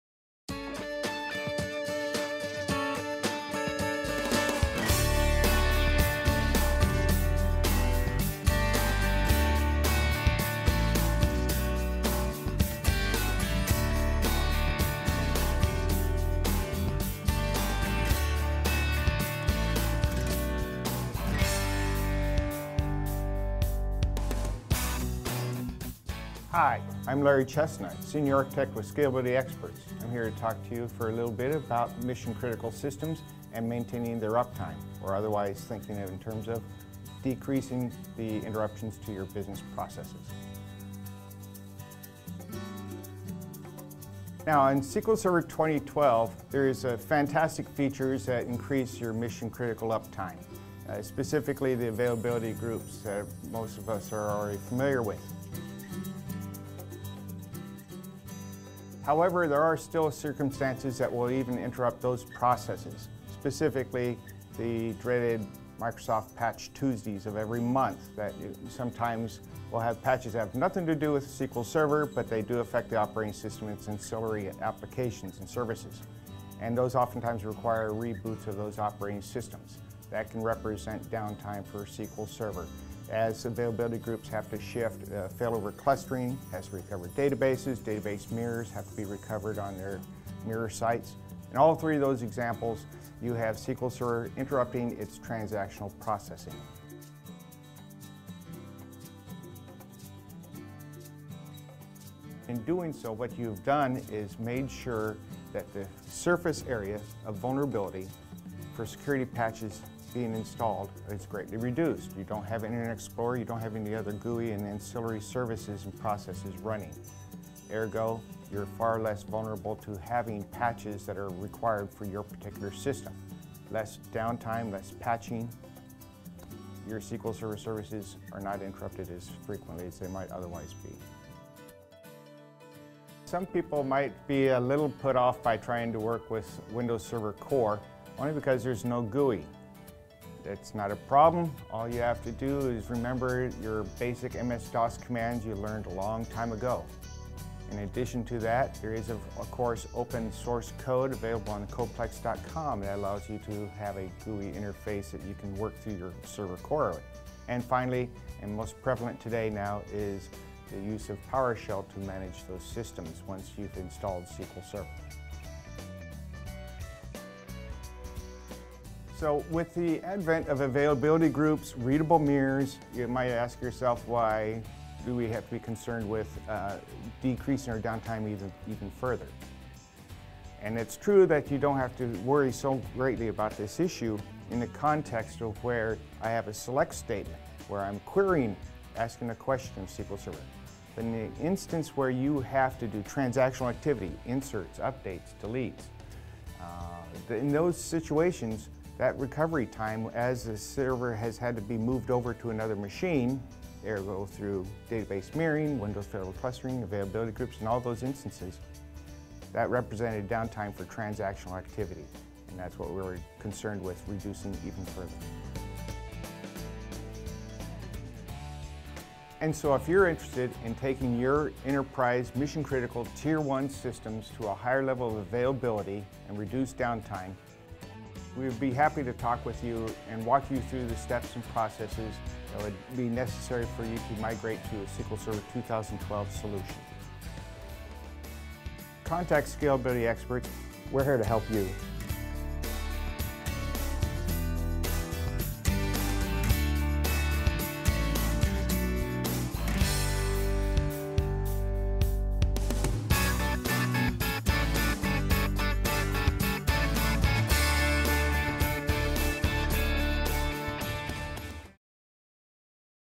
Achieving High Availability with SQL 2012 and Windows Server Core - Scalability Experts
Senior Architect Larry Chesnut, with Scalability Experts comments on how to achieve high availability with SQL Server 2012 and Windows Server Core.